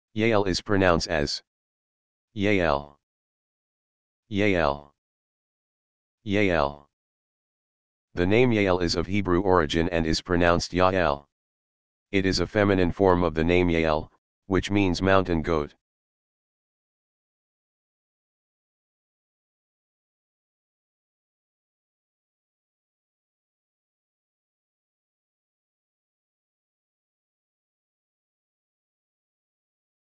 How To Pronounce Yael
The name Yael is of Hebrew origin and is pronounced "yah-EL". It is a feminine form of the name Yael, which means "mountain goat".